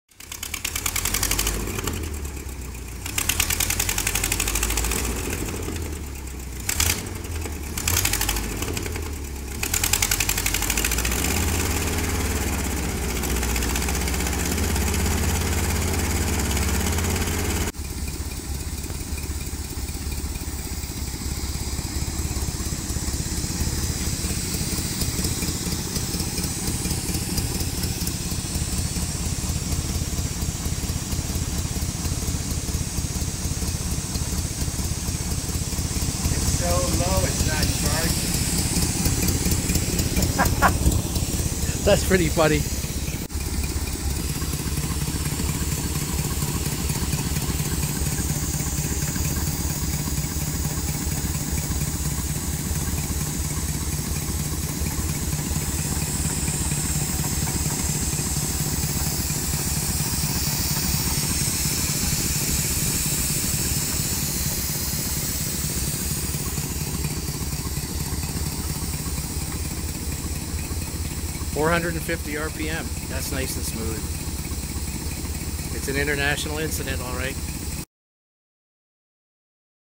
International Scout V8 HEI Duraspark Conversion - 450 RPM Idle - Part 8
Idling smoothly at 450 RPM... so low that the Delco alternator isn't charging!  Converted obsolete  "Gold" ignition module and stock distributor altered to fit Ford Duraspark pickup and cap, powering a GM style TBI coil with a GM HEI module, with a GM three wire alternator!  Cheaper by far, and more reliable by far too.